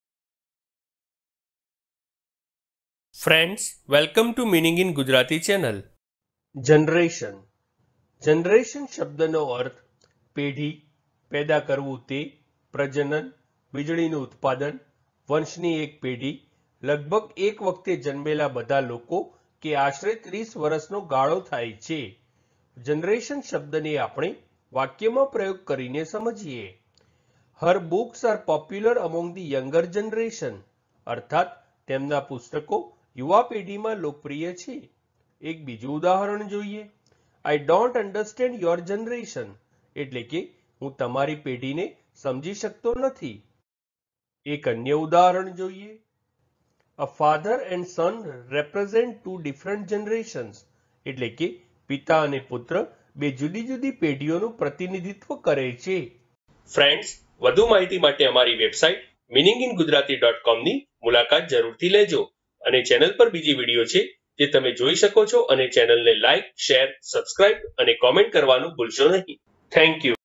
Generation Meaning in Gujarati | Generation નો અર્થ શું છે | Generation in Gujarati Dictionary
આ વિડિયો માં તમે Generation નો Gujarati માં અર્થ સમજશો અને એની સાથે Generation નું pronunciation પણ શિખશો. એટ્લે કે તમે Generation ના meaning ની સાથે સાથે એ પણ શિખશો કે Generation ને કેવી રીતે બોલાય, Generation ને બોલવાની સાચી રીત કઈ છે.

તો બસ એક મિનટ માં શીખો Generation શબ્દ ને.

Generation ને examples થી સમજીએ